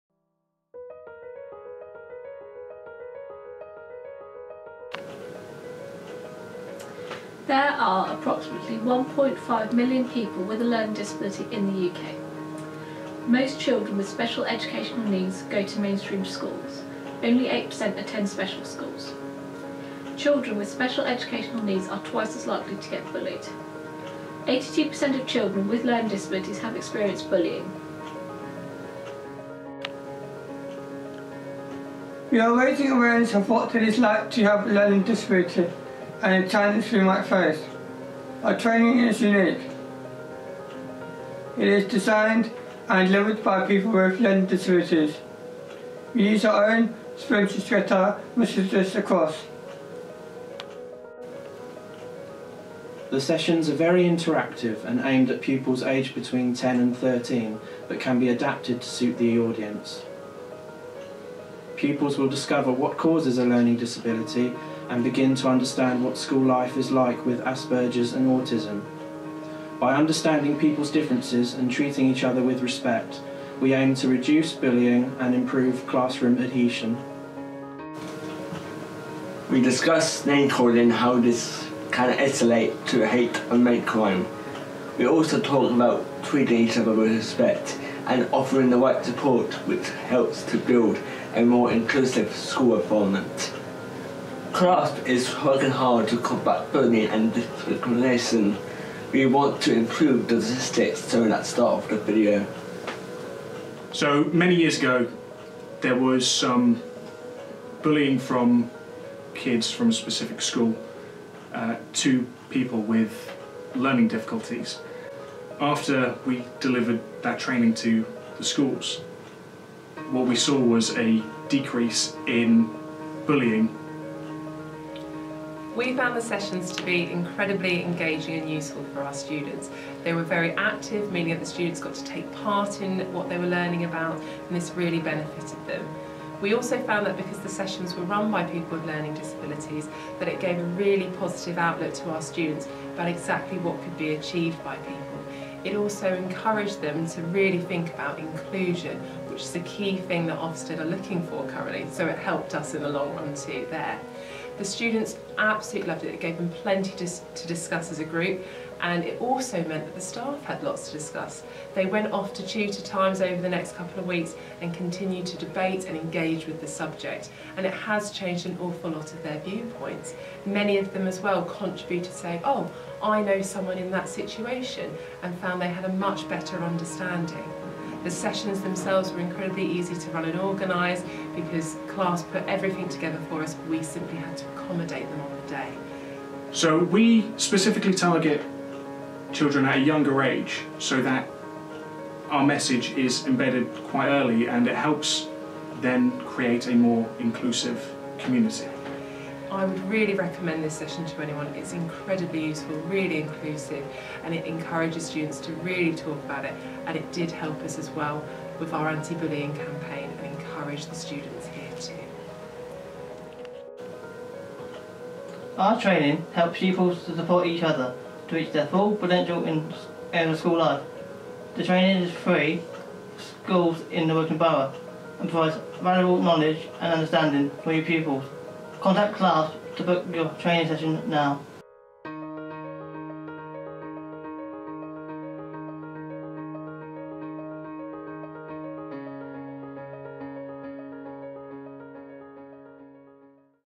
CLASP Learning Disability Awareness Training for Schools
CLASP is a self advocacy charity for adults with learning disabilities in the Wokingham Borough. Our training group, Listen To Us, have created a Learning Disability Awareness workshop which is free for schools in the Wokingham Borough.

This video tells you what the training covers, testimonials and information on how to book your session.